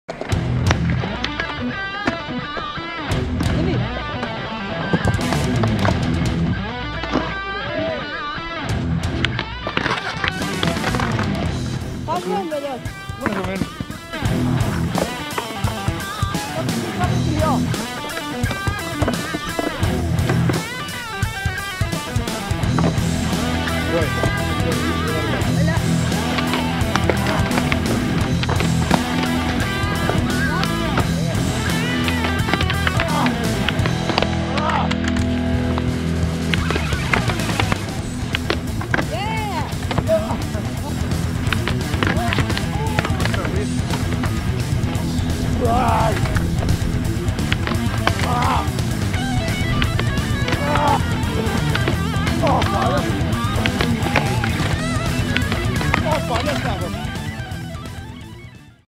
Skatada #82 (Trailer)
Mais uma pequena batalha, duma manobra que nem sequer se tentava há uns tempos valentes. será que saiu? 🤨
Resposta no próximo episódio...   😅